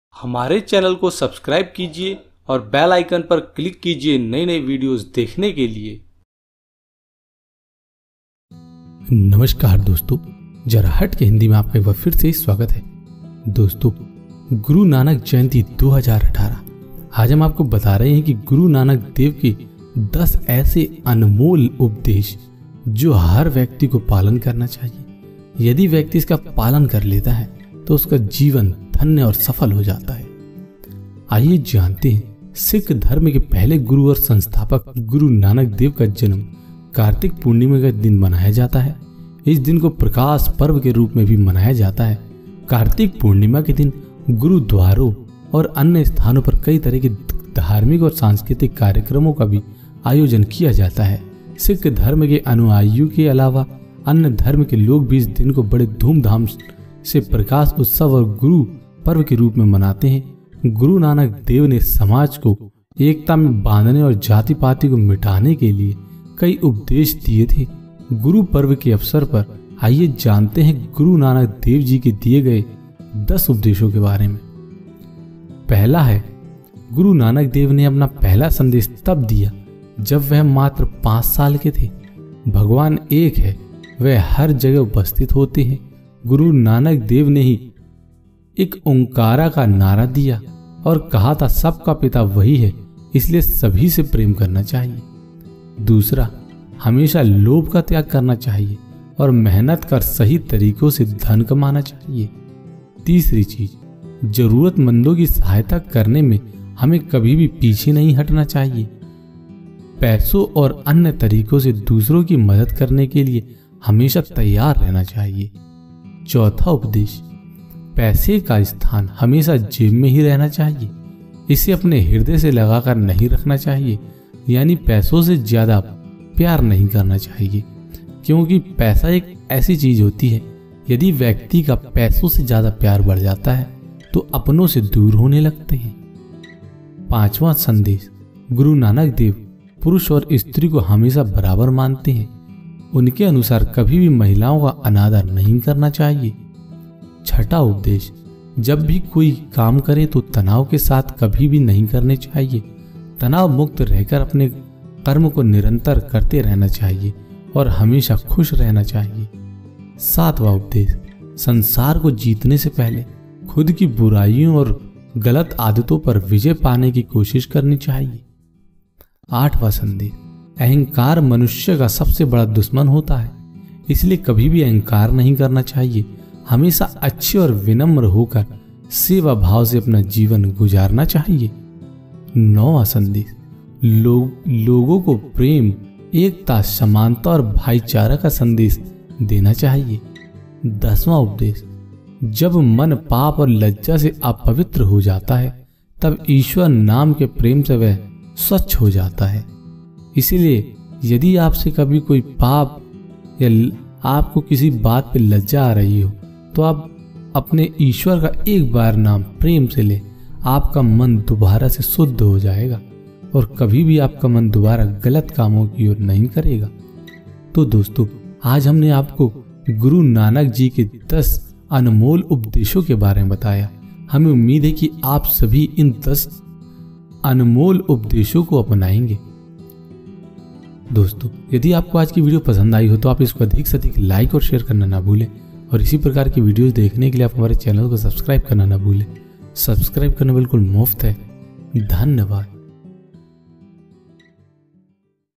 गुरुनानक जयंती 2018: गुरु नानक देव के इन 10 अनमोल उपदेशों को यदि पालन कर लिया तो जिंदगी बदल जाएगी
चाणक्य नीति पुस्तक खरीदने के लिए नीचे क्लिक करें :